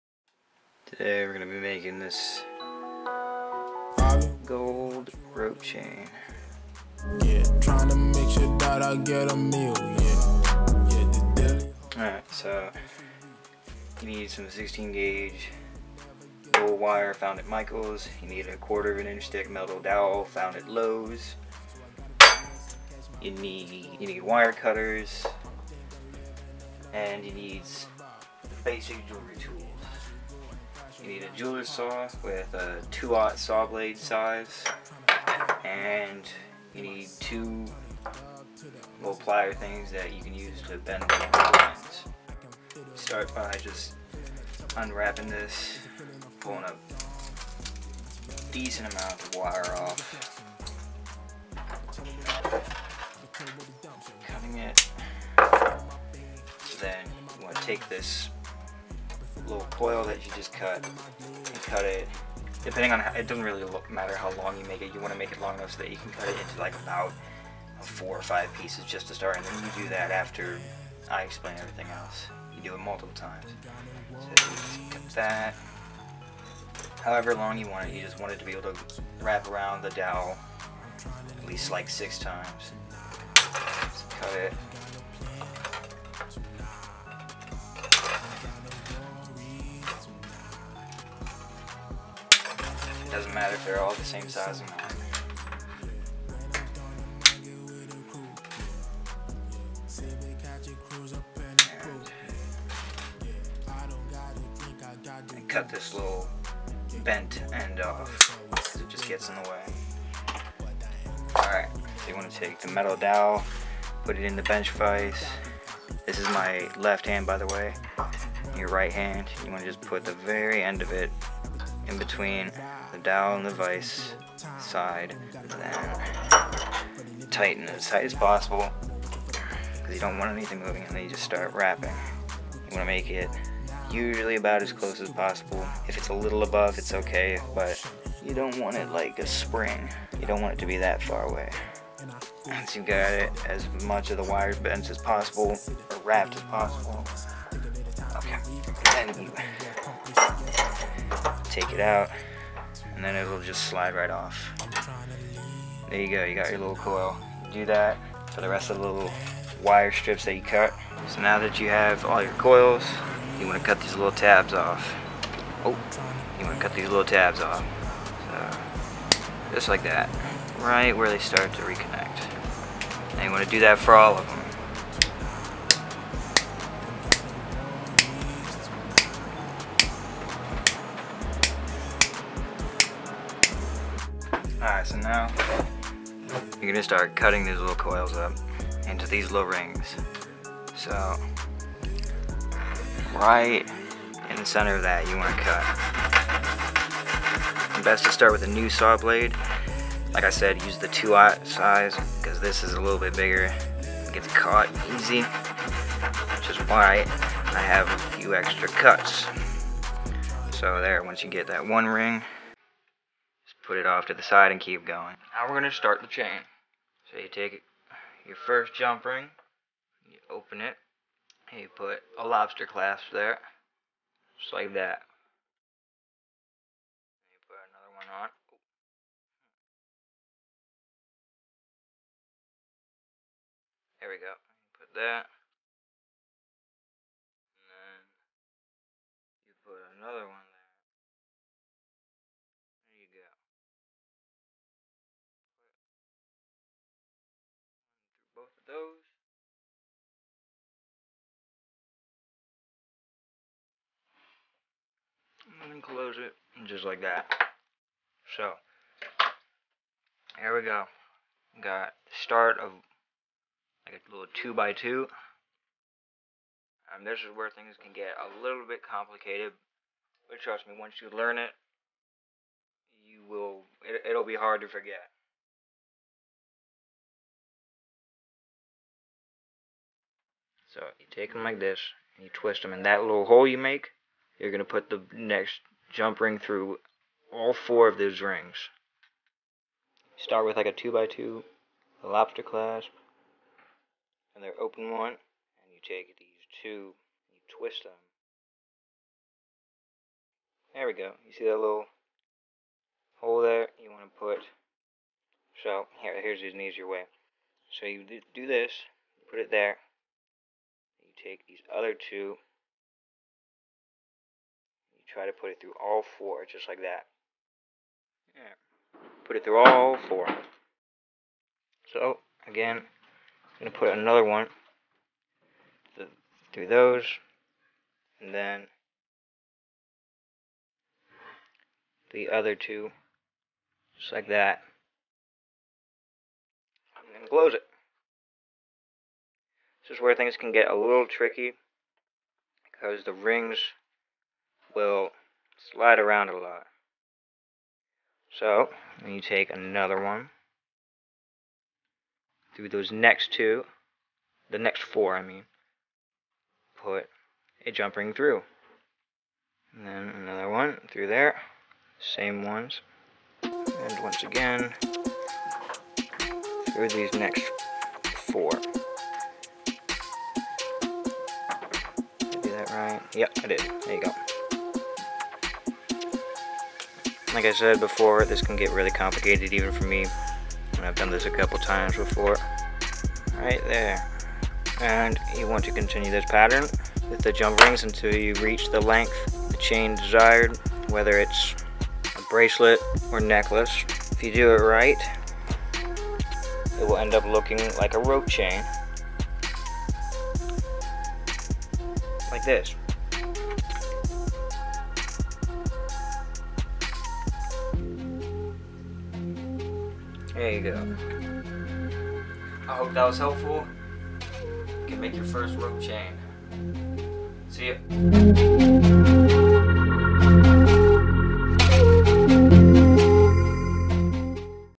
How to make a Rope Chain (no solder)
How to make a smooth, sleek rope chain with 16 gauge wire from Michael's without soldering anything.